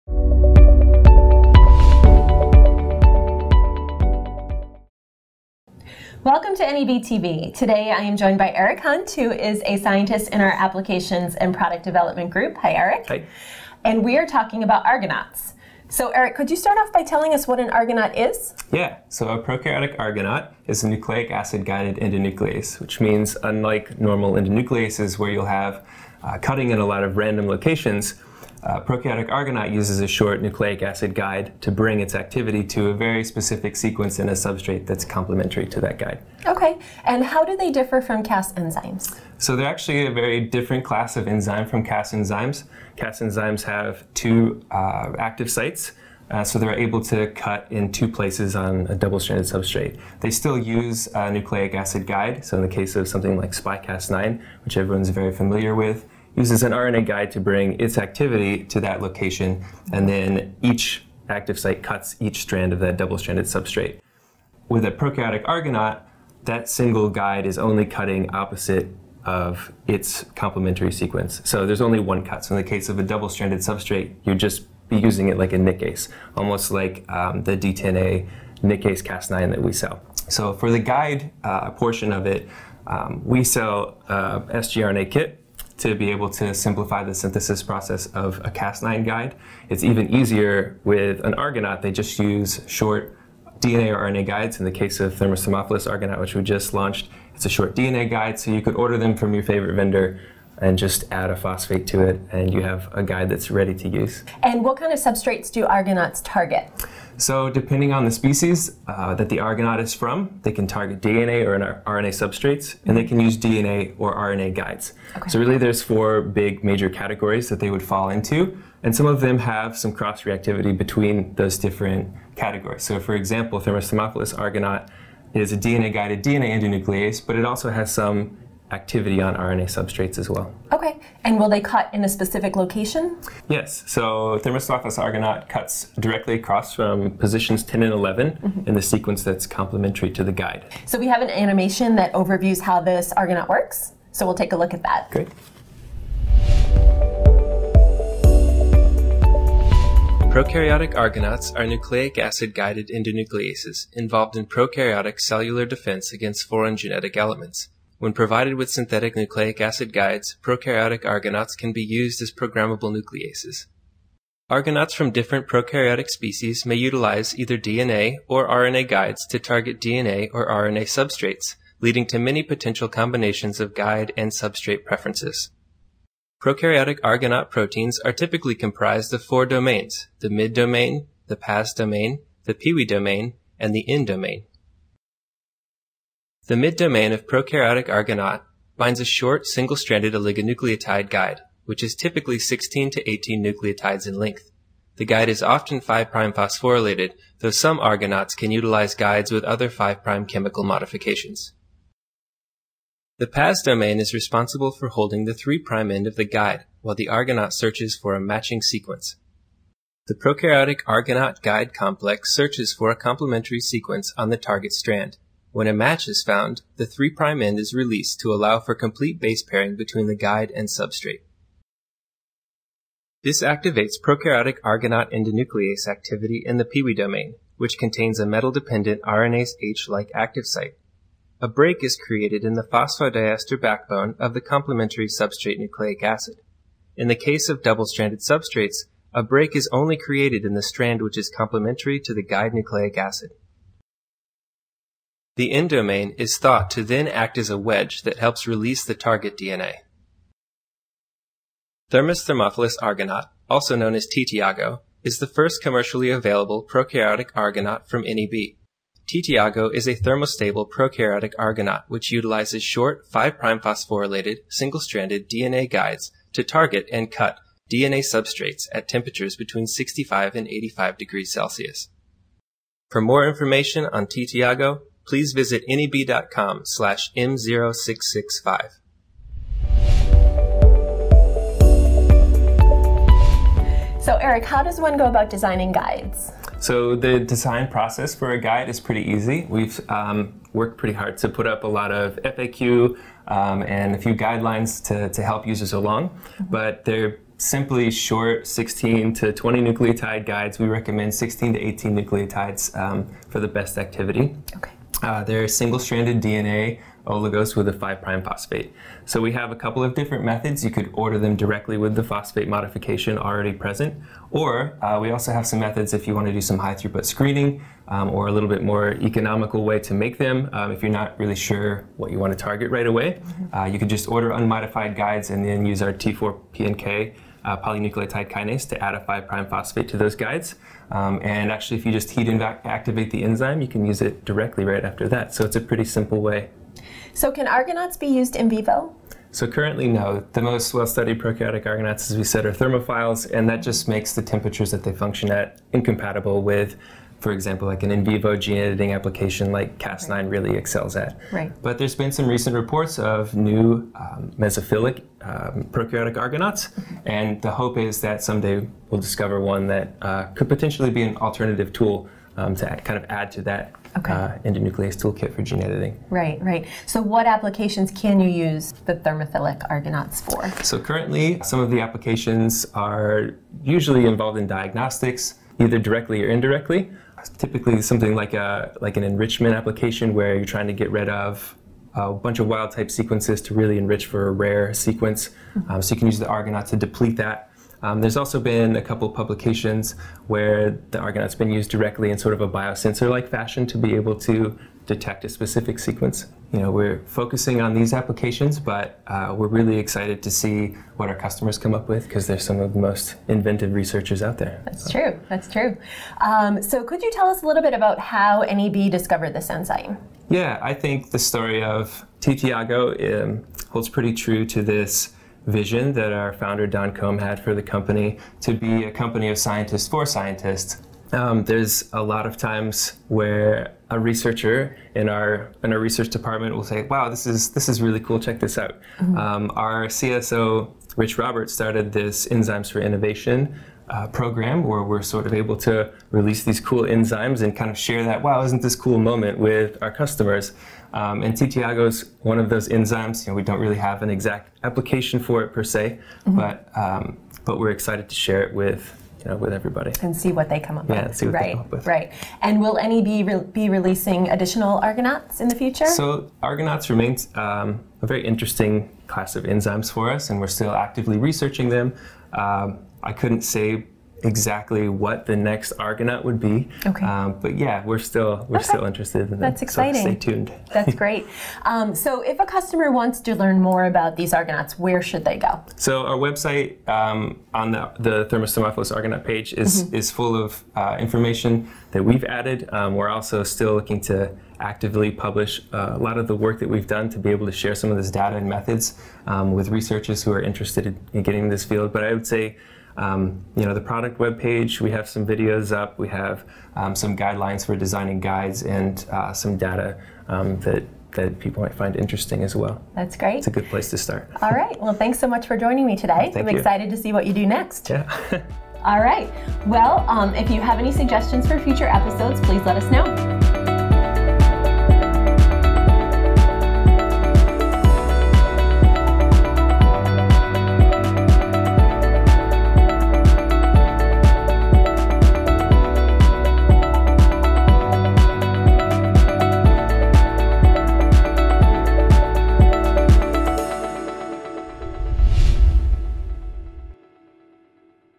NEB TV Ep. 31 -What are Argonaute Proteins?
In this episode, learn more about argonautes, including how these nucleic acid-guided endonucleases differ from Cas enzymes and potential applications.